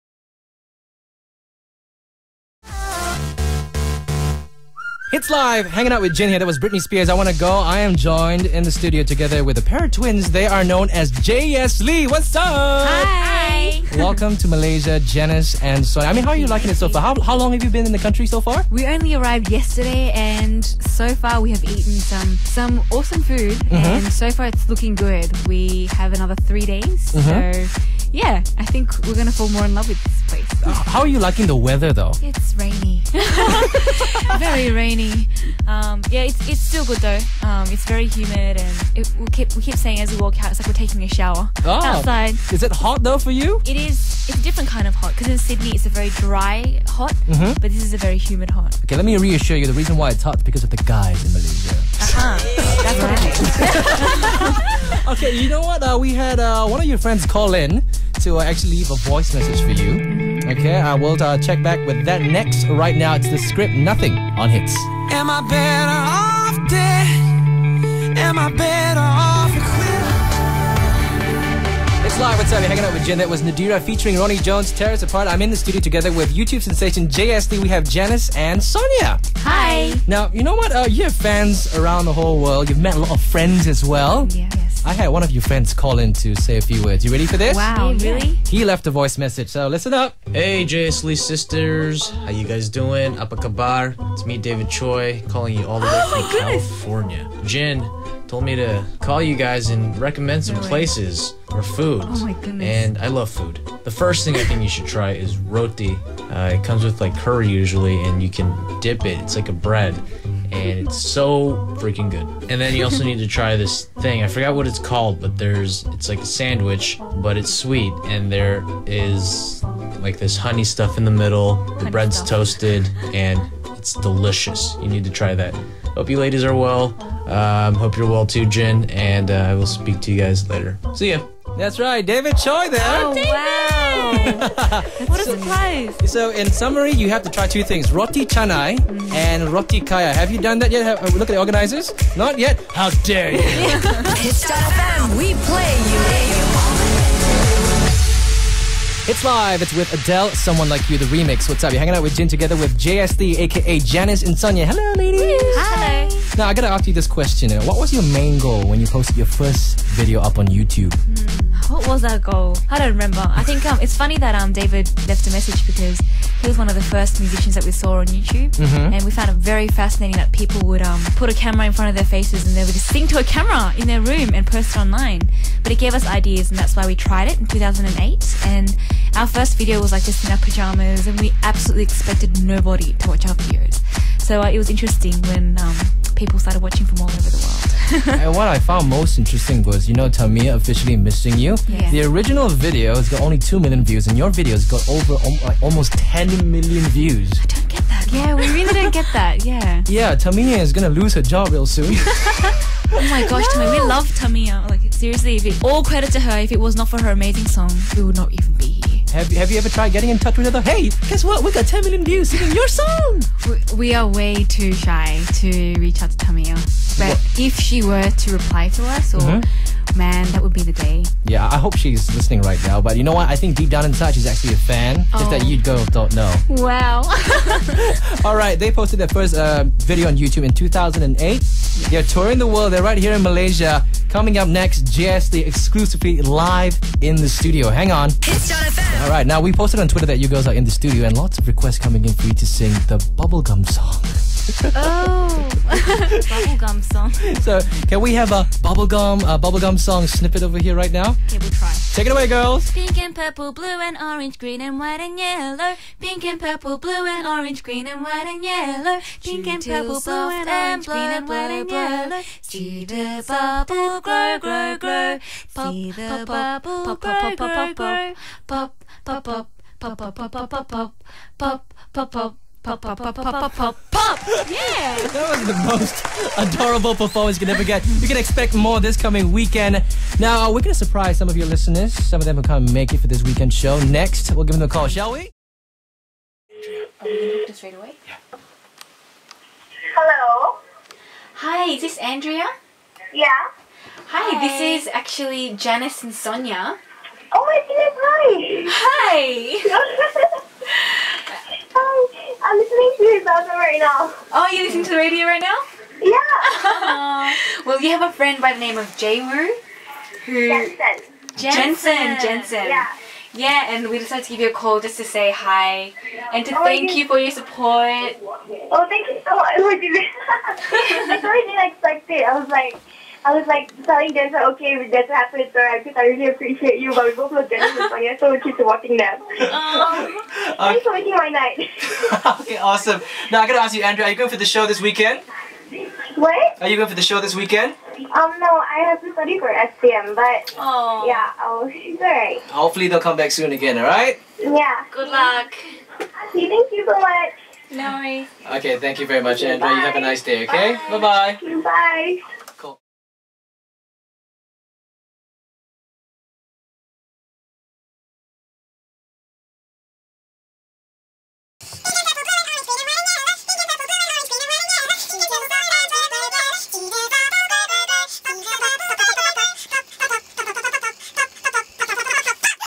Jayesslee's Exclusive Interview on hitz.fm
Jayesslee dropped by the hitz.fm studio for an exclusive interview with Jin, and an exclusive meet and greet session with their fans.

David Choi also made a special appearance on the show!